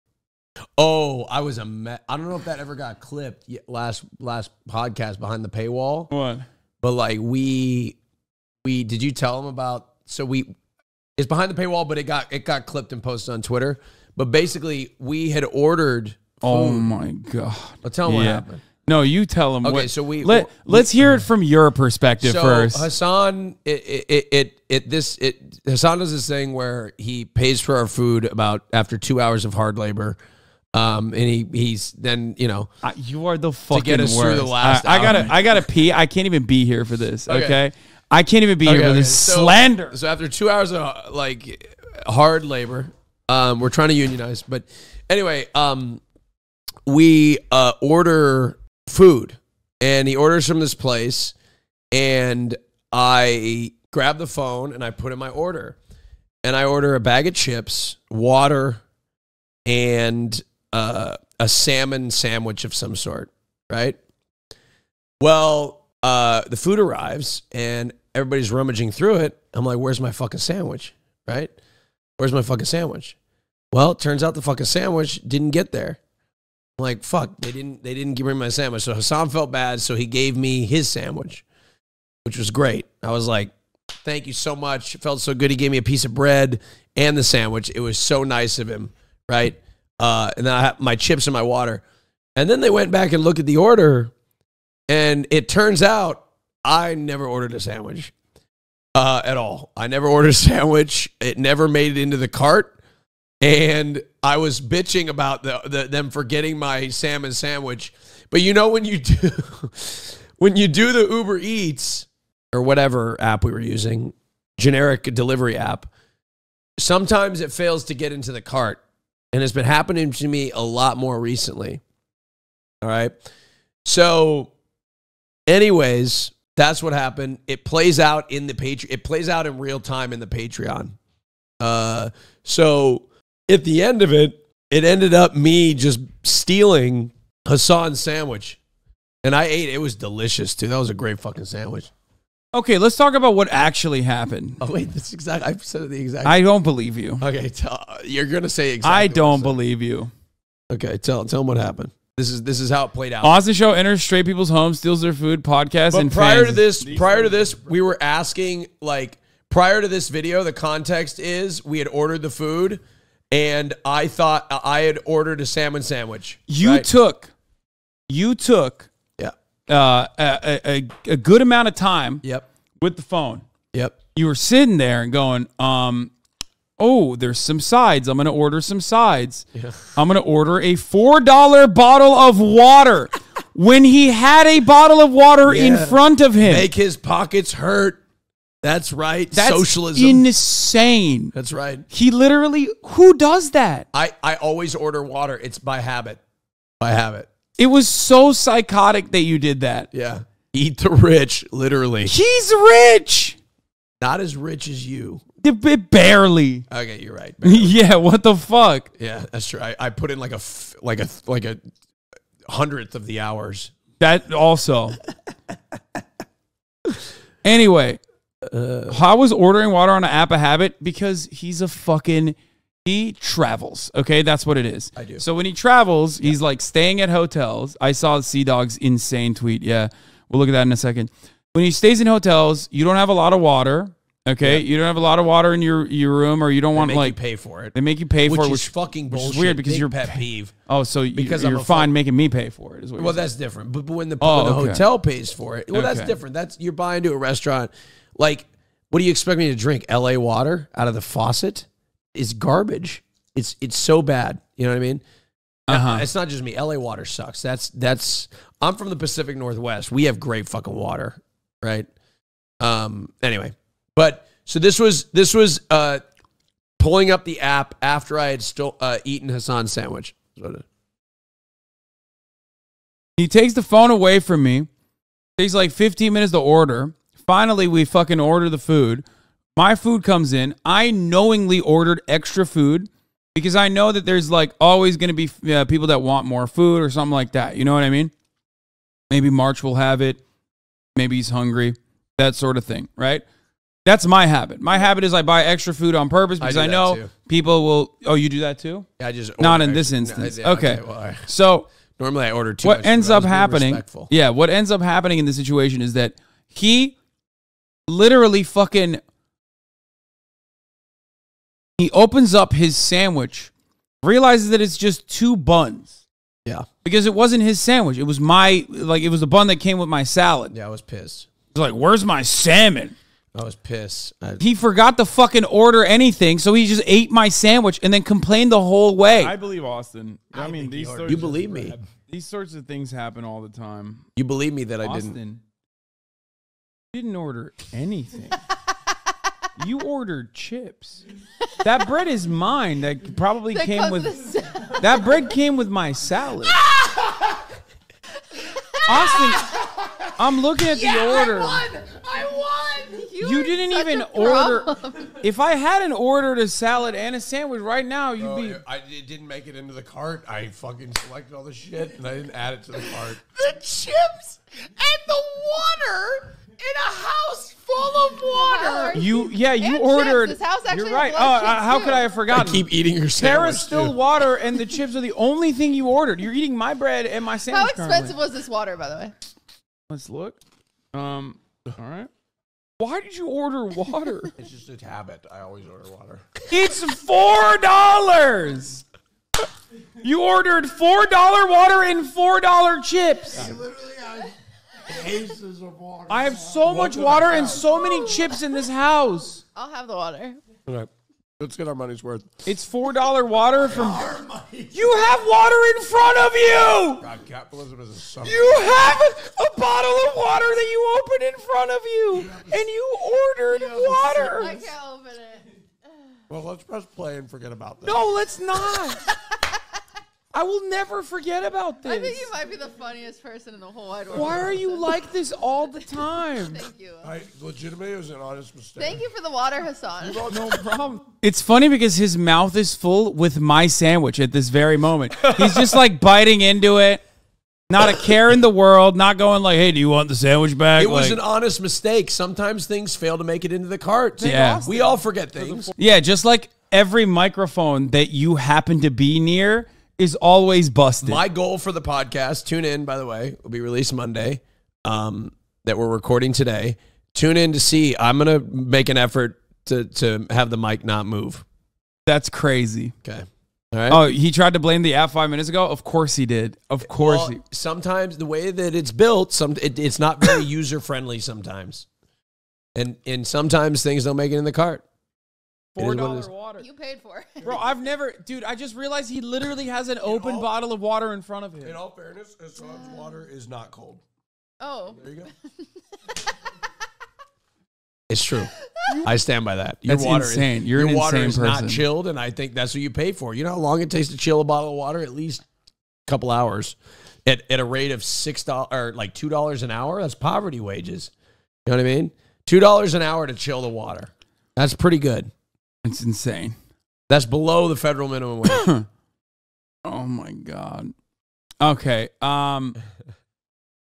HasanAbi roasts Austin for stealing his sandwich
This is a fan channel.

Hasan Piker is a Turkish-American Twitch streamer. You can find him at his official channel here: